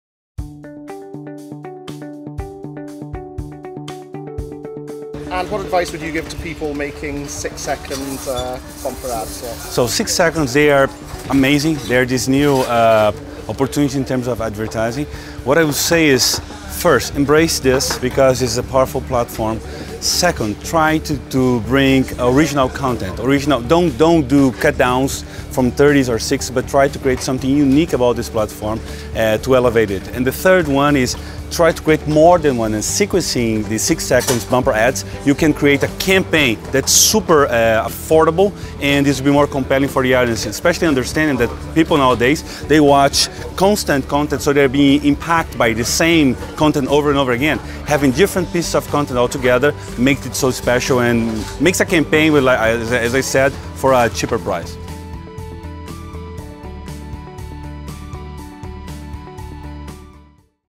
Campaign Cannes diaries 2017 - Renato Fernandez
Renato Fernandez chief creative officer at TBWA/Chiat/Day Los Angeles, talking about how to use those 6-second ads.

The video was taken with Xperia XZ Premium
@SonymobileME and Sonyxperiame